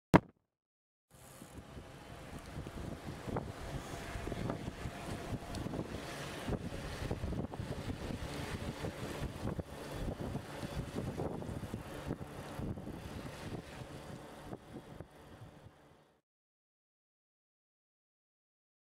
Taster 2 - Culprit Junior 2 road bike
Short taster of footage taken while filming the Culprit Junior 2 road bike review at Redbridge Cycling Centre